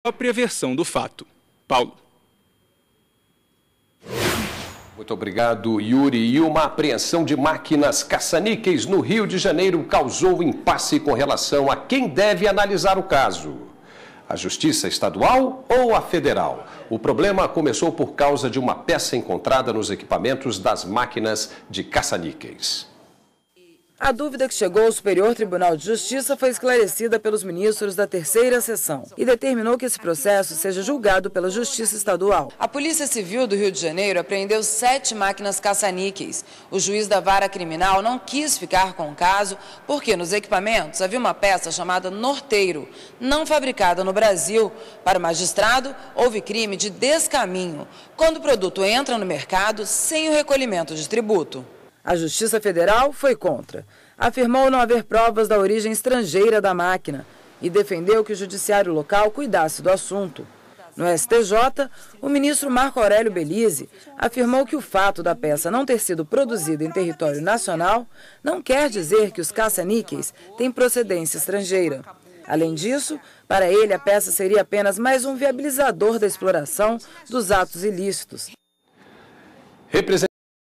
Caça-níqueis (11/10/12)
Uma apreensão de máquinas caça-níqueis, no Rio de Janeiro, causou impasse com relação a quem deve analisar o caso: a Justiça Estadual ou Federal? O problema começou por causa de uma peça encontrada nos equipamentos das máquinas de caça-níqueis.